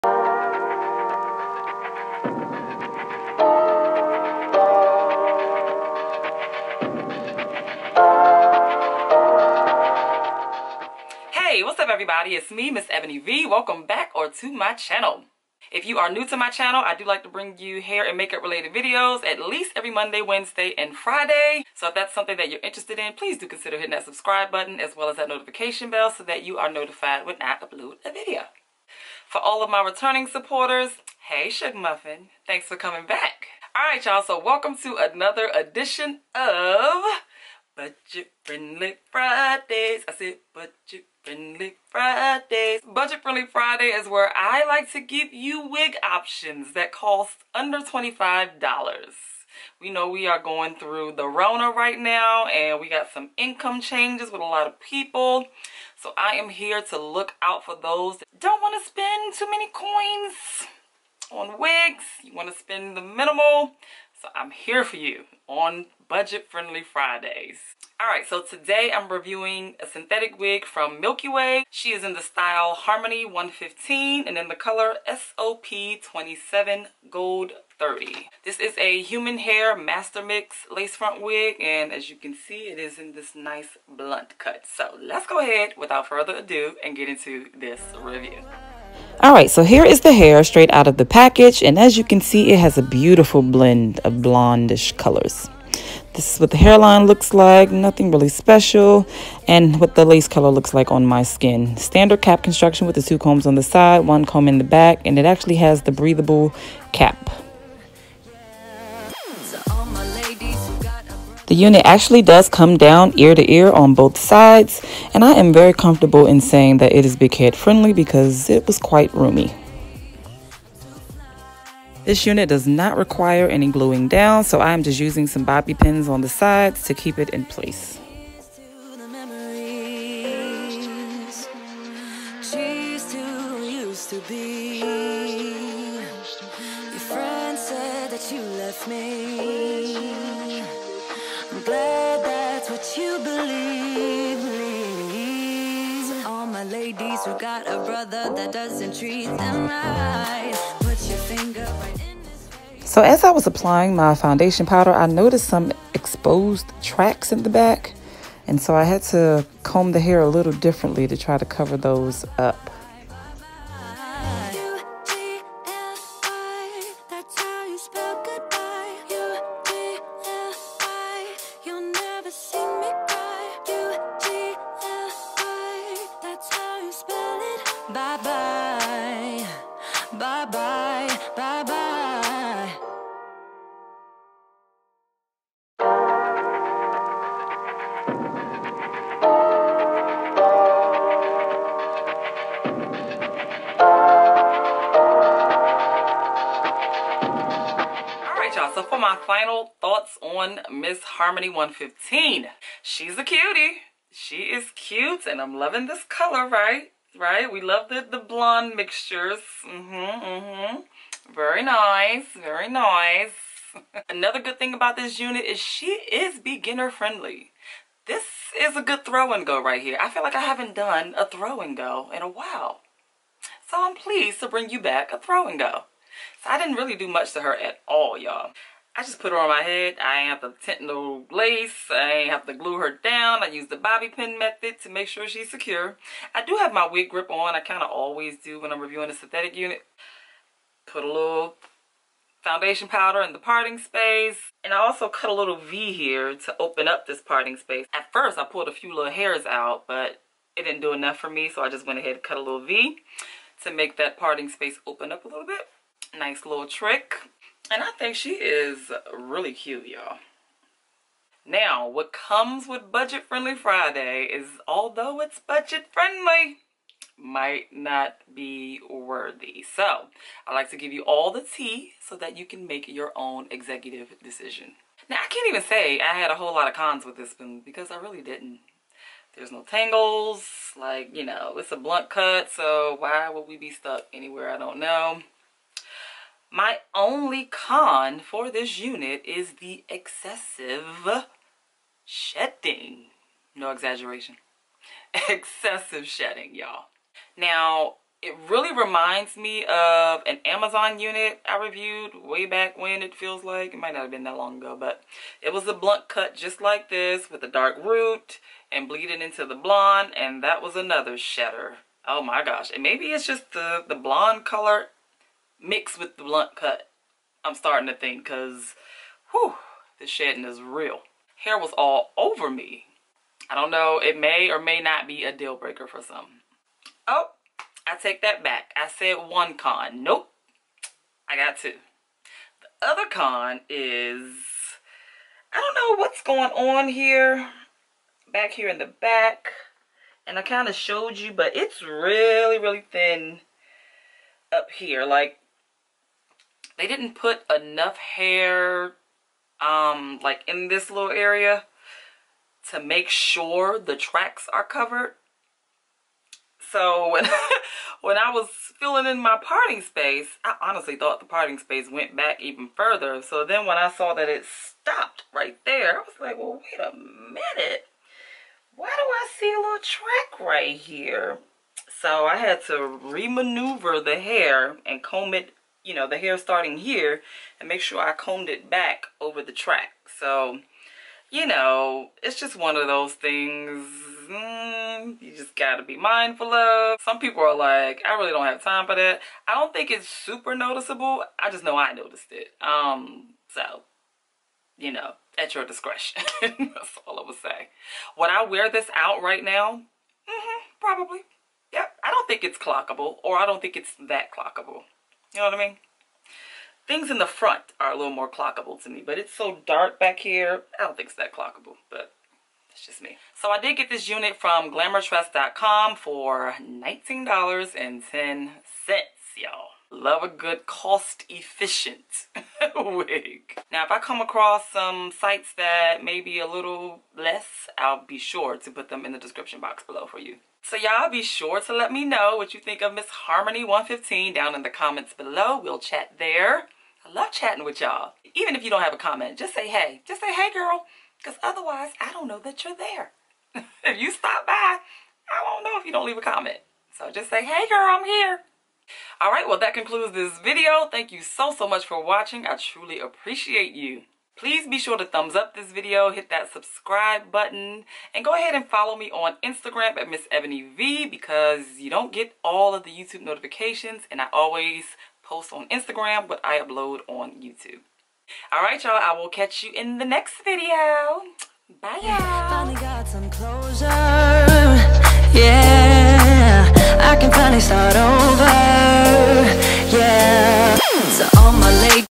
Human Hair Blend Blunt Cut Bob – Budget Friendly Friday (Ep.5) – Milky Way HARMONY 115
Hey Everybody,

Check out this human hair blend blunt cut bob from Milky Way in the style Harmony 115 and in the color SOP27GD30.  This is another edition of budget friendly Fridays!

Video Schedule:
Mondays – Human Hair Reviews
Wednesday – Synthetic Wig Reviews
Fridays – Budget Friendly Fridays (Wig Reviews under $25)

*Links to Hair*
Milky Way Human Hair Blend Lace Front Wig Harmony 115 - $19.10

HAIR SPECS:
COLOR:  SOP27GD30
CAP SIZE:  Medium
LACE:  Brown Lace Front
TEXTURE:  Silky
LENGTH:  12”
PARTING:  3” Lace Part
SHEDDING:  Heavy
TANGLING:  None
DENSITY:  130%
OVERALL RATING:  7/10